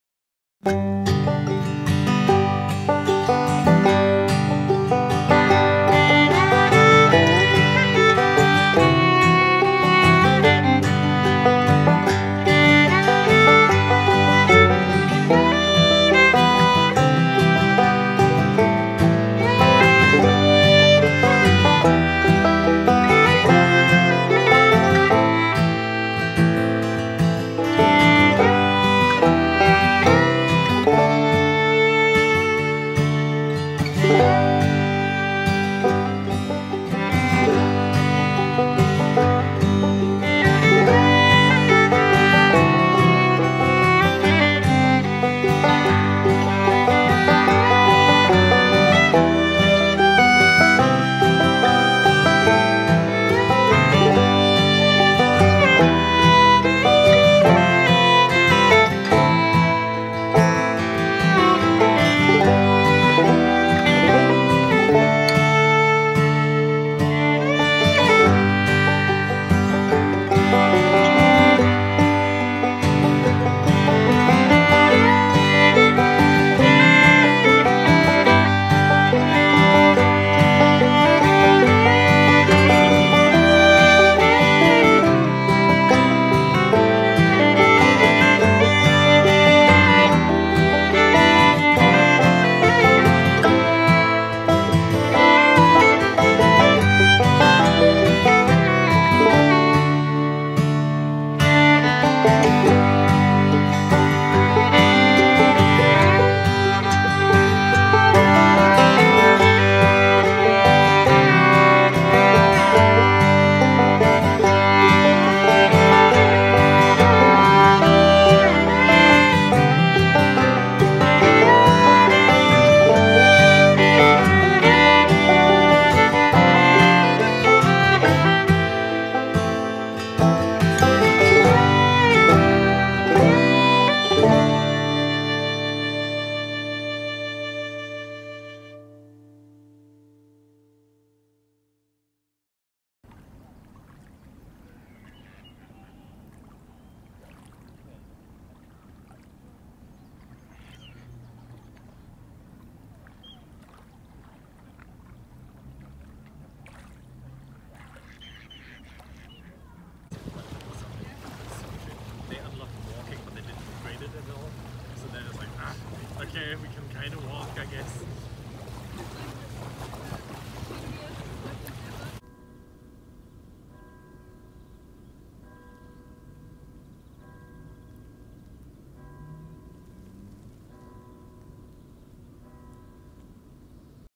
Peaceful lake view in Switzerland | Lugano
In this video you can see a peaceful lakeshore with full of birds.